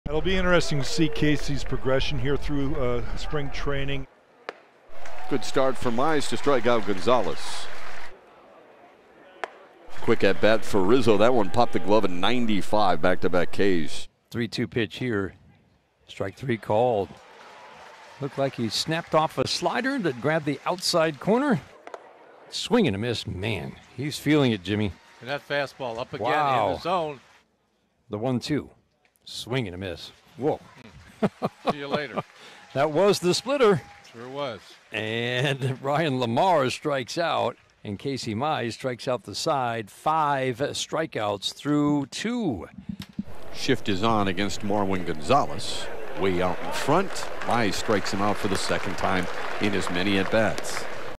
Detroit Tigers Casey Mize Pitches 3 Inning, Striking Out 6 Batters
Detroit Tigers Casey Mize goes 3 Innings, striking out 6 batters in Spring Training.